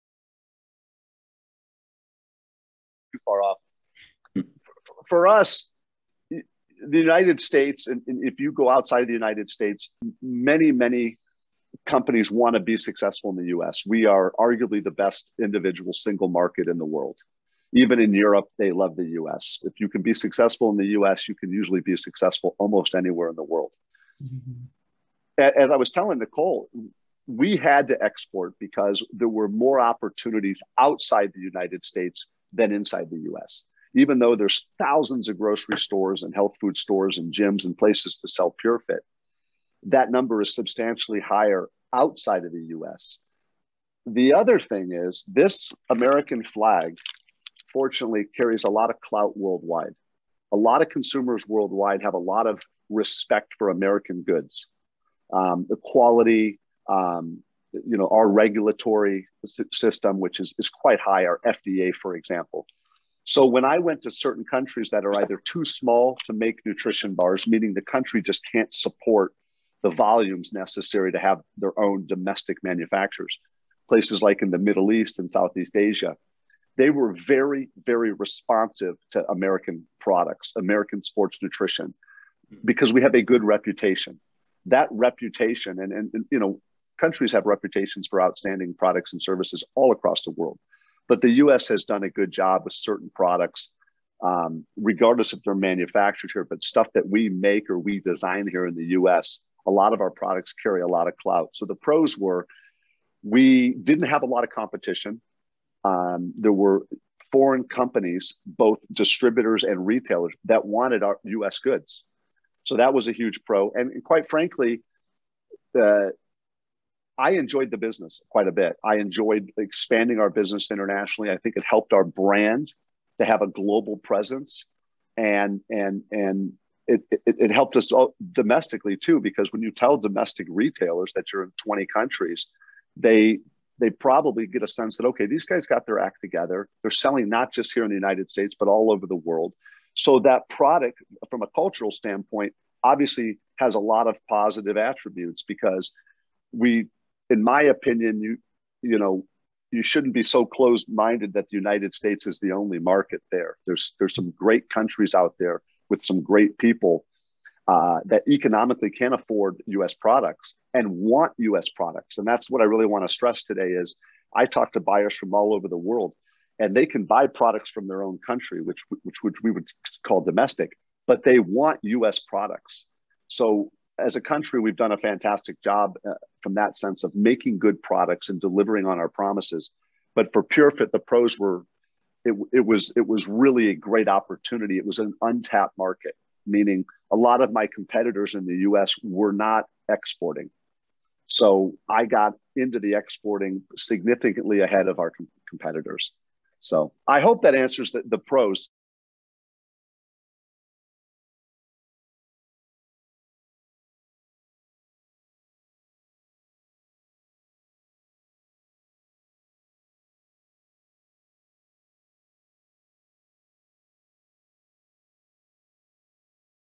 Doing Business Overseas Speaker Series: Pros of Exporting/Importing for PureFit
Robb Dorf, CEO of PureFit, joined Nicole Kelly from Global Trade/Bay Region/
Cañada College and Wesley Alexander from CoBiz Richmond, Inc. to share his thoughts on navigating opportunities abroad for small business. We were truly honored to learn from Robb who is insightful and passionate about paving it forward, so please take a listen and enjoy.

Robb Dorf, BS, Exercise Physiology, Founder & CEO